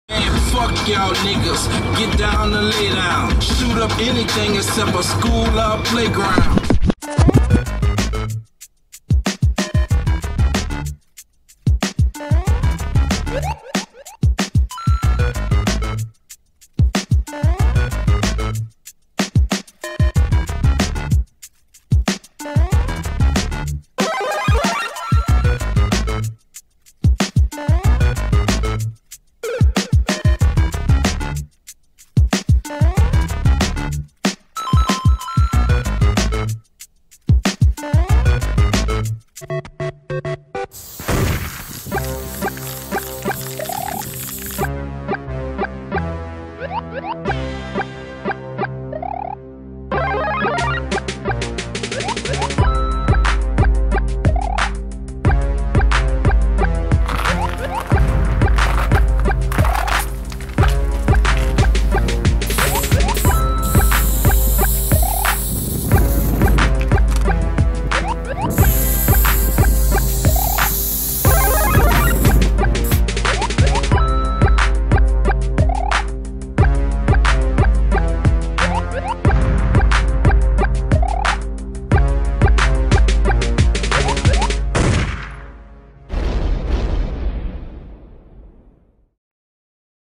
[GRAFFITI] Super mario x ROAD
Super mario graffiti piece done today.. Yes that's snow on the ground; caps fucked up, cans fucked up, piece kind of fucked up, but not to bad so I post it anyway. Hope you like it.

music and soundtrack belongs to the artists and their record label.